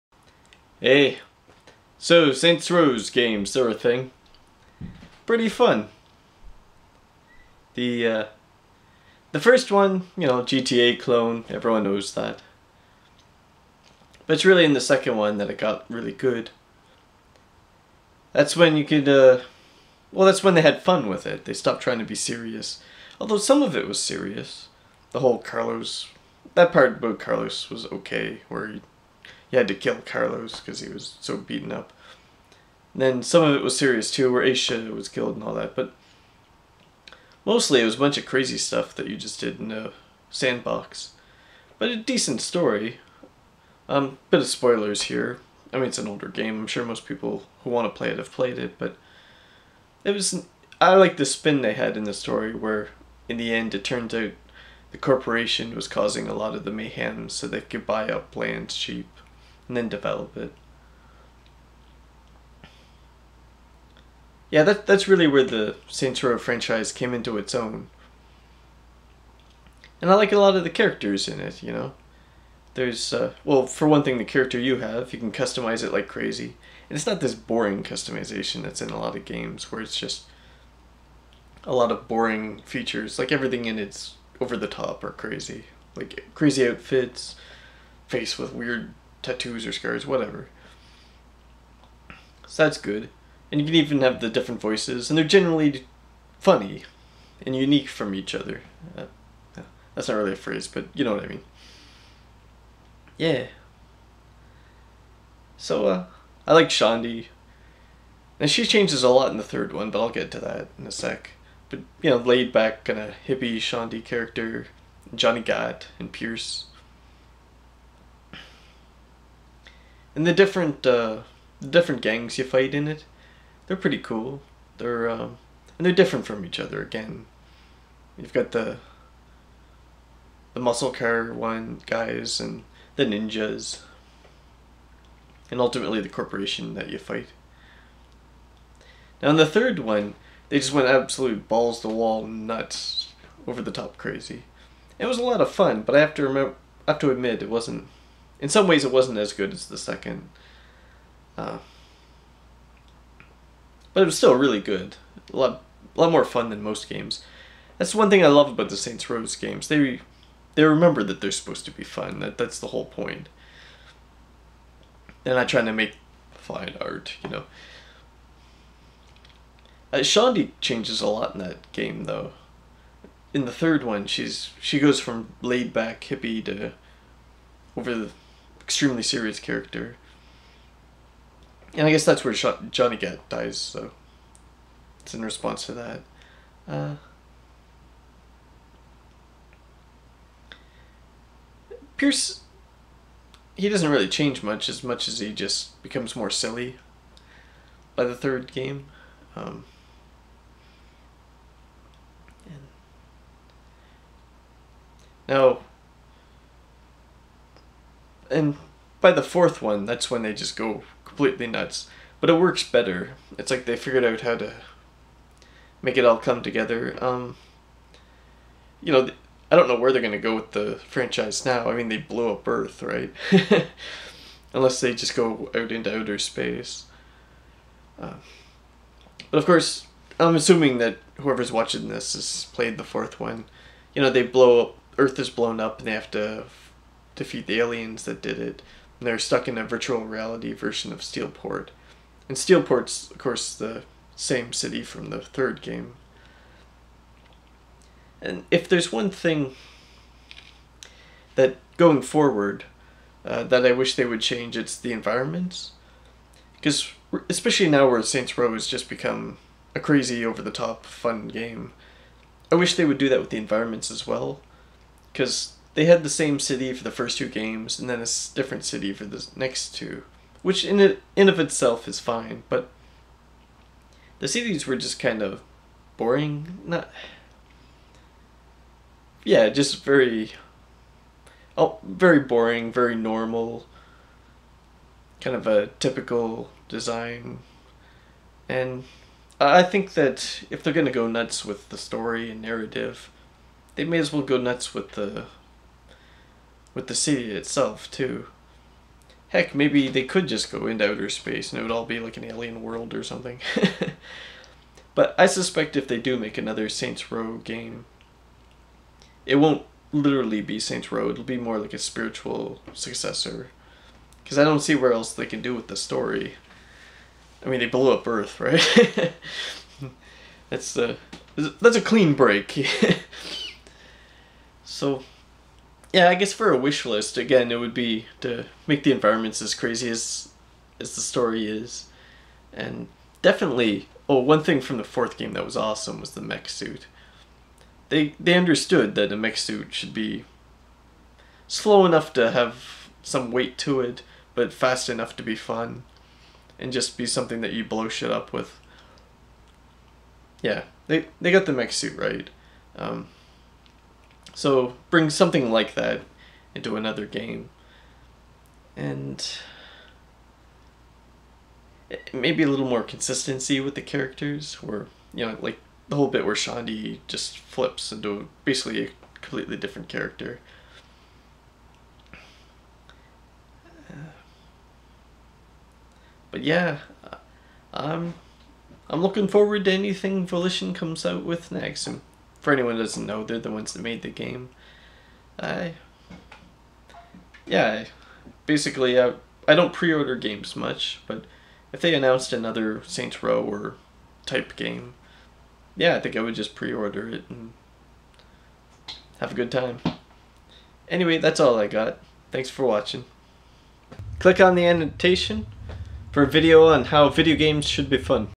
Saints Row Memories and Wishlist
Chatting about Saints Row games and what I would like to see in a sequal.  These video games are fun and I would like to see more stufff like it in the future.